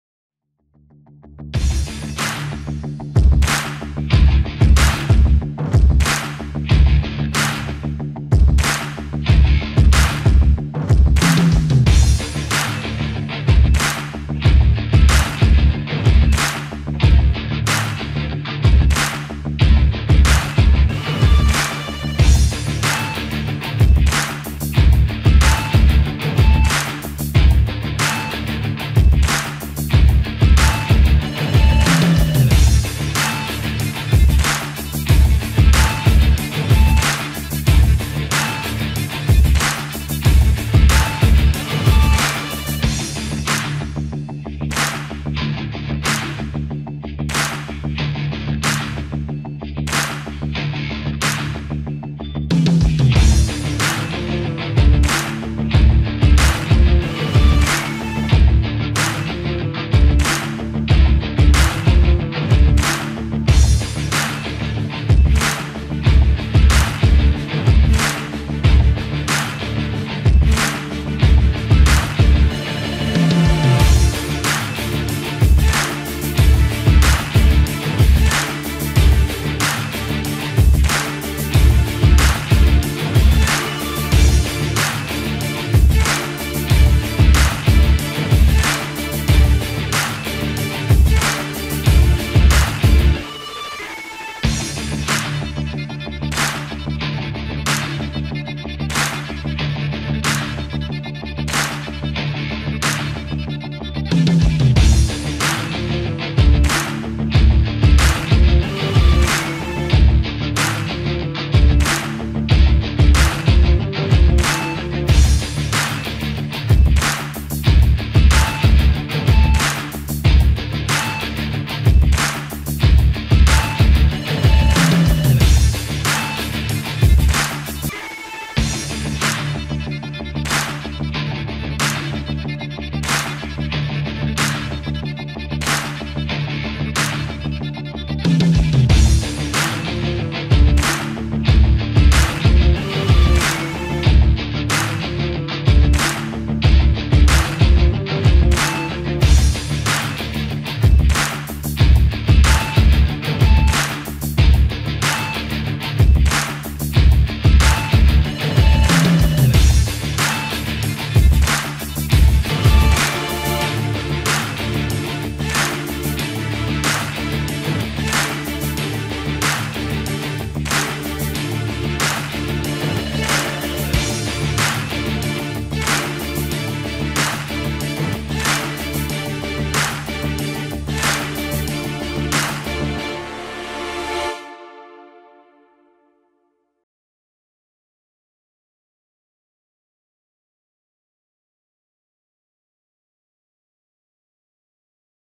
An indian flag 3D Effect in photoshop
how to ceate  or make an indian 3D flag easily in photoshop